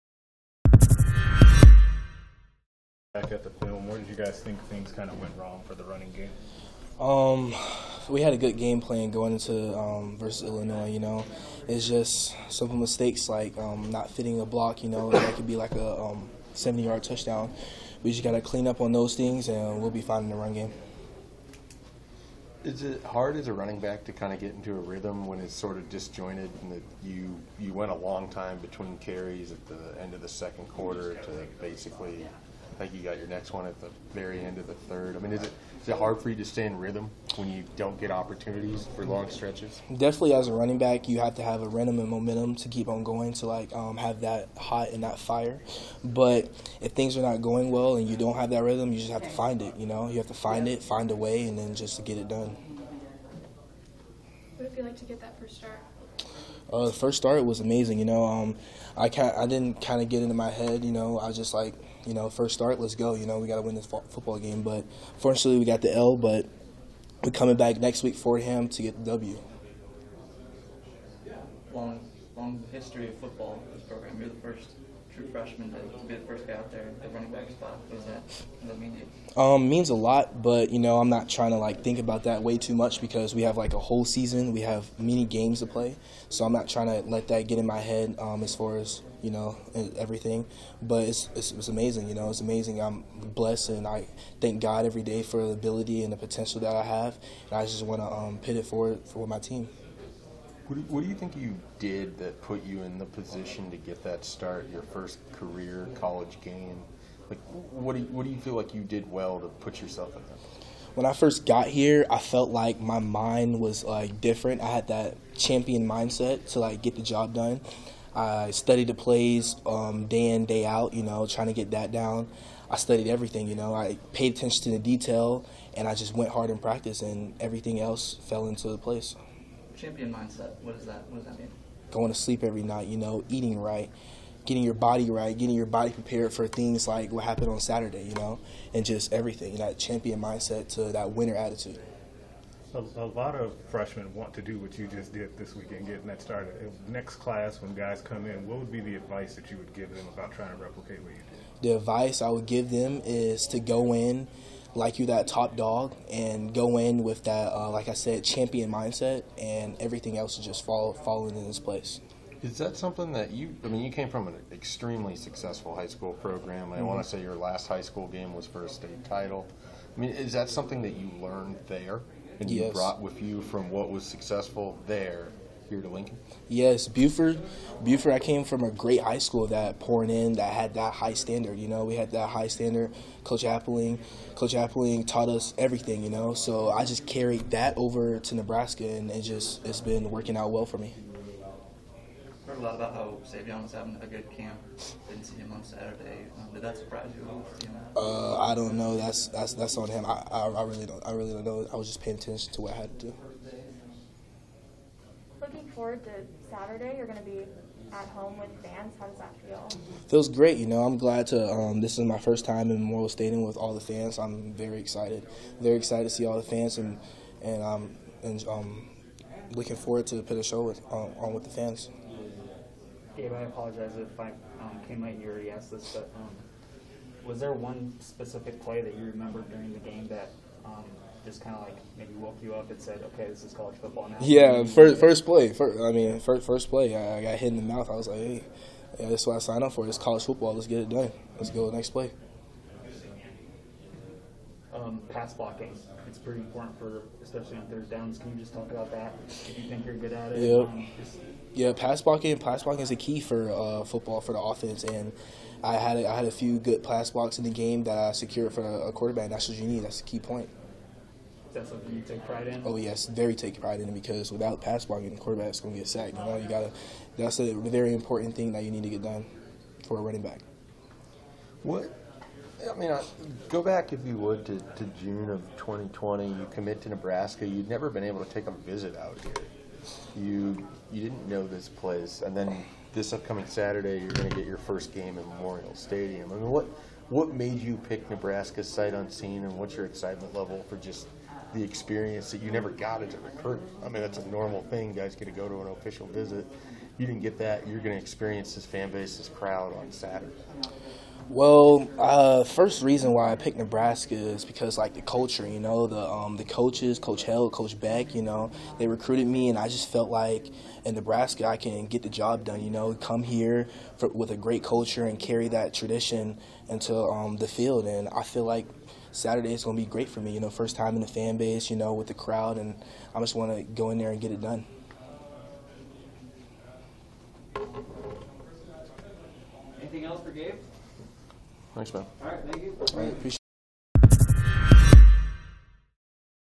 HOL HD: Gabe Ervin Jr. Monday Press Conference
Nebraska Running Back Gabe Ervin Jr. meets with the media on Monday following practice.